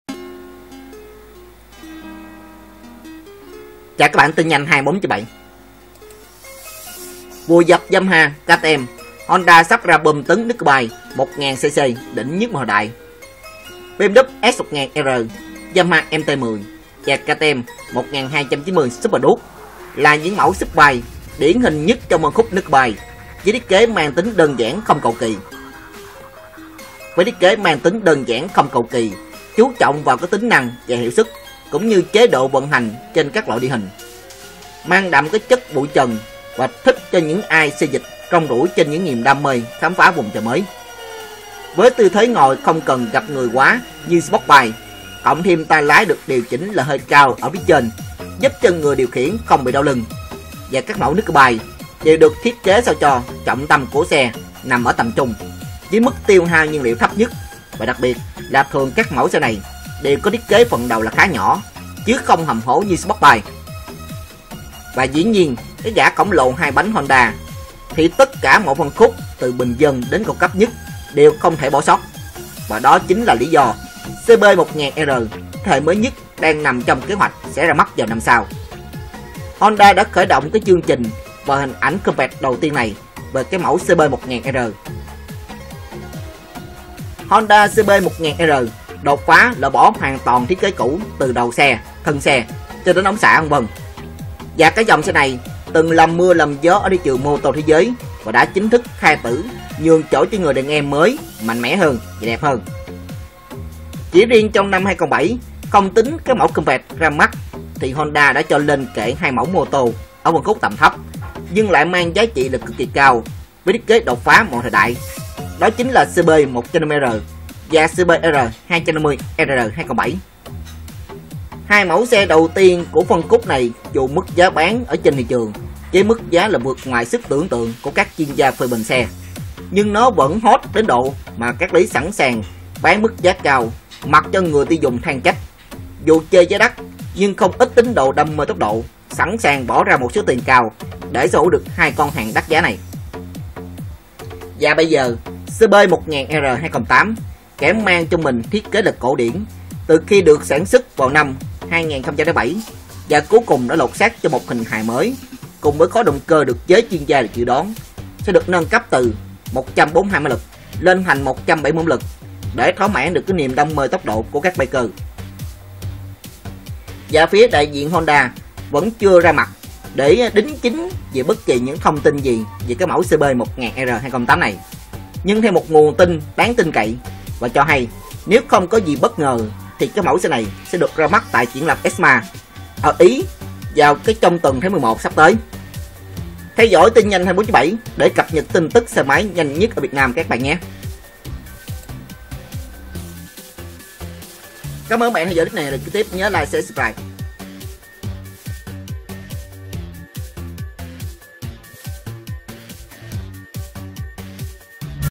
Tin nhanh 24/7 - Vùi dập Yamaha ,KTM Honda sắp ra Bom tấn Nakedbike 1000cc đỉnh nhất mọi thời đại.
BMW S1000R, Yamaha MT-10, và KTM 1290 Super Duke là những mẫu SuperBike điển hình nhất trong phân khúc NakedBike với thiết kế mang tính đơn giản không cầu kỳ.